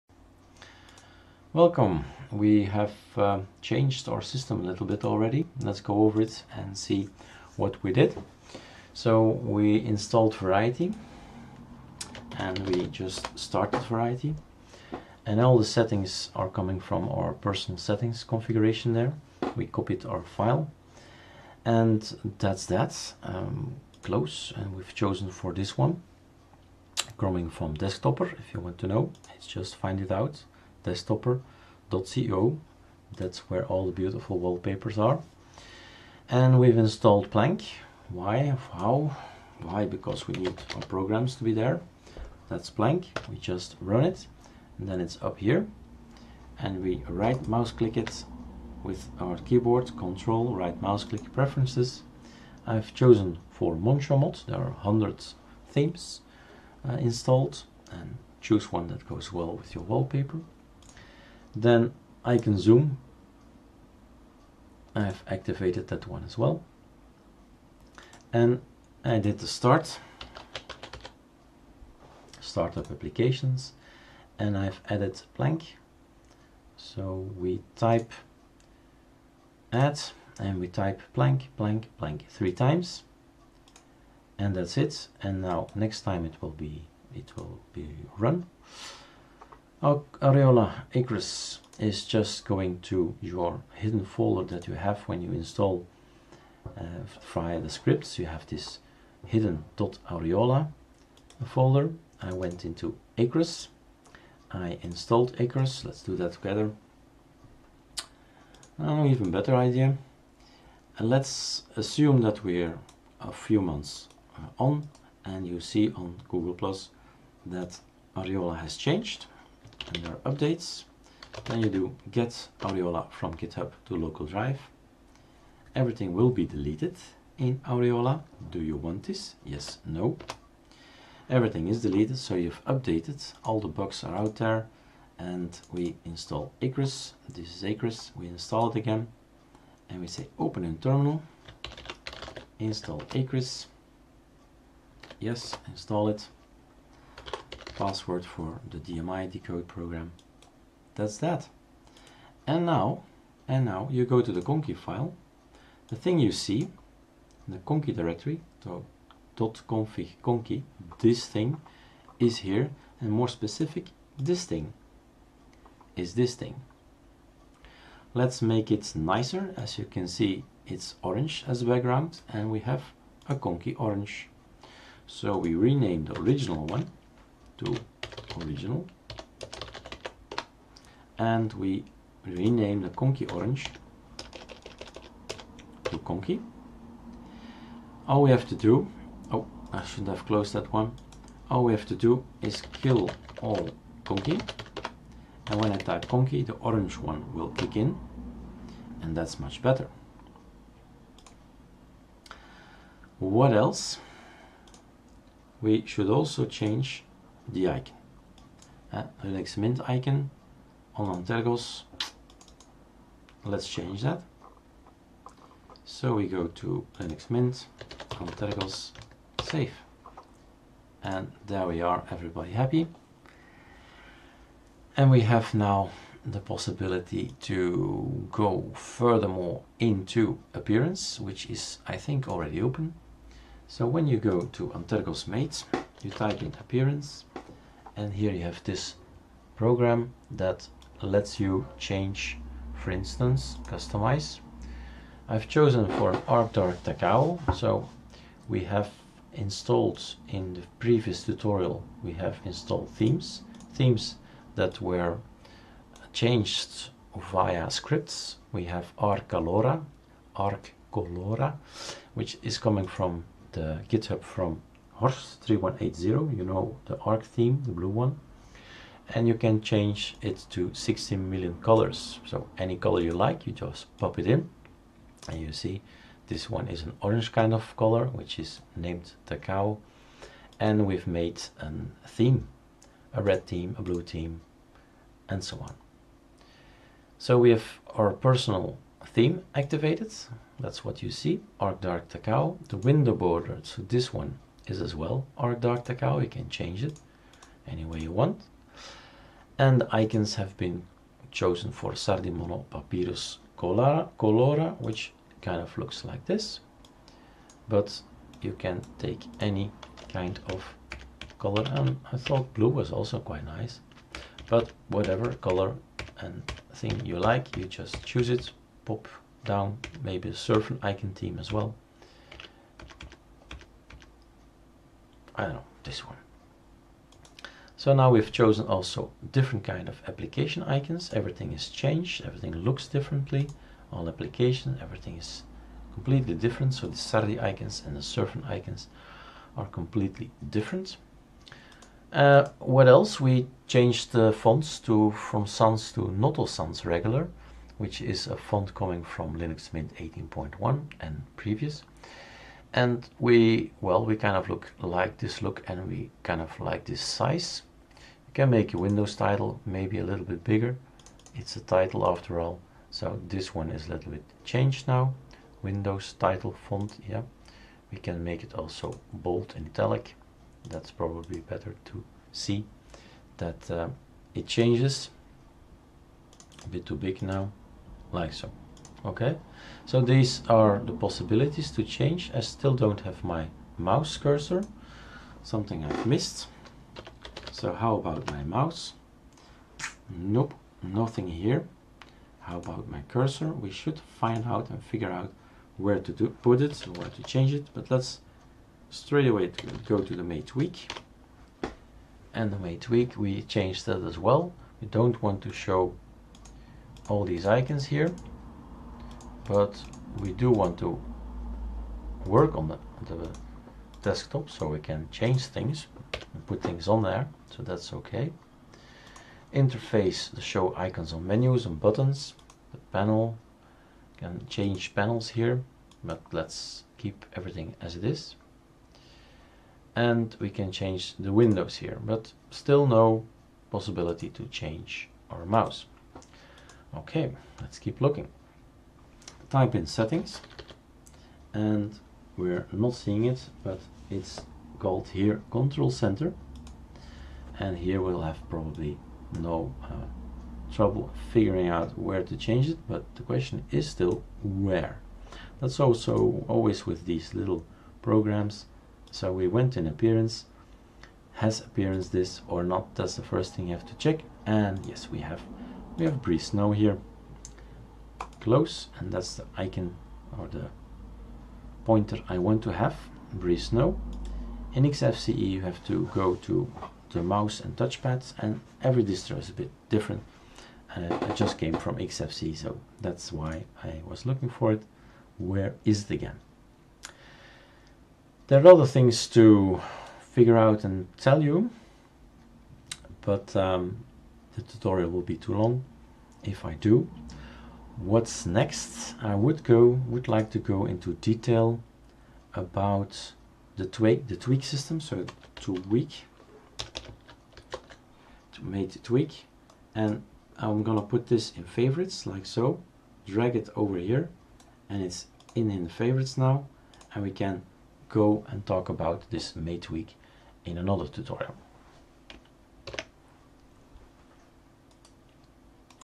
How to install Antergos Mate and customize it - 3 customization of Antergos Mate
We have chosen to install the mate desktop on Antergos and we will show you how to customize it.